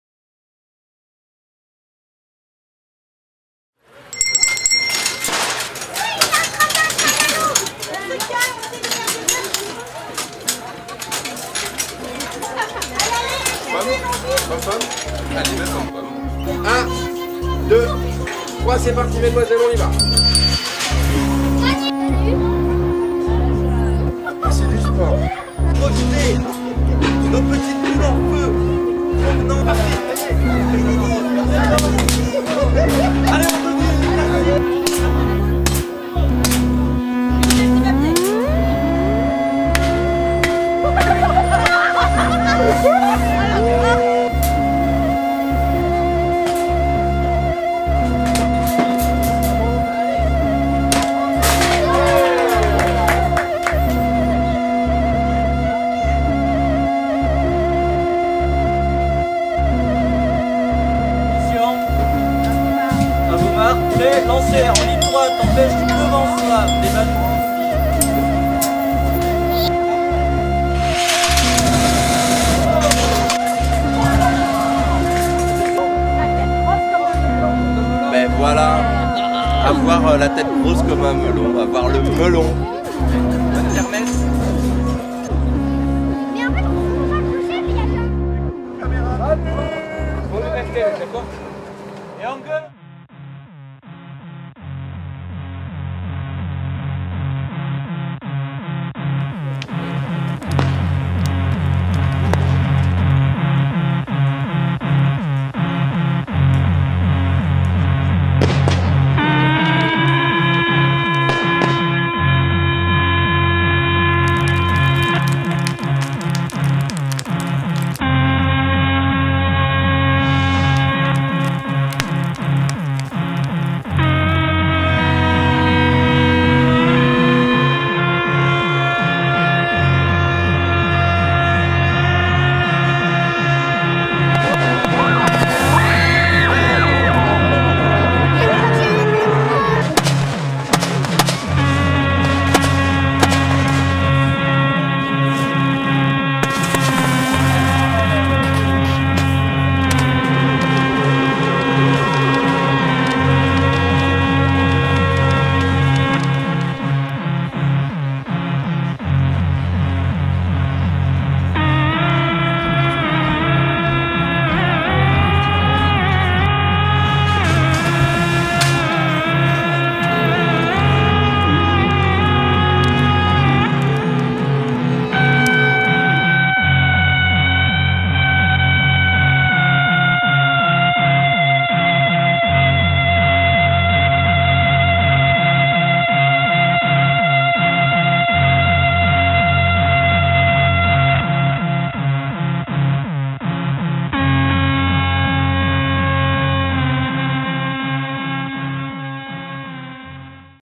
La Kermesse & Flammes
Durant Les Odyssées, Festival d'Ambès édition 2013, les caméras de Station Ausone ont capturé quelques images de cette soirée magique.

Musique de Emilien Leroy.